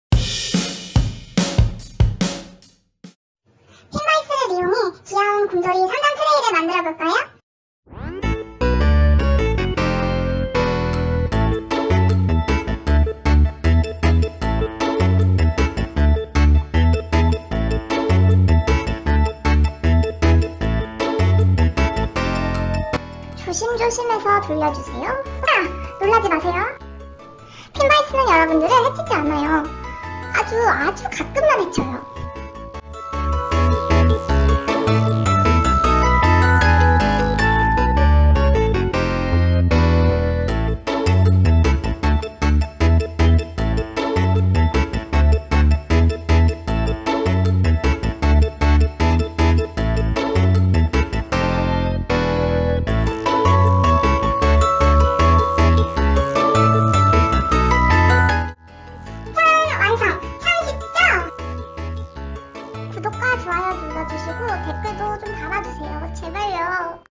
[미니토끼의 만들기 무료강좌] 핀바이스와 몰드를 이용해 삼단트레이를 만들어요~
(주)미니를사랑하는사람들 소속 작가들의 영상 커뮤니티 채널입니다.

핀바이스와 몰드를 이용해 귀여운 곰돌이 삼단트레이를 만들었어요
예쁜 디저트와 함께 세팅해 보세요^^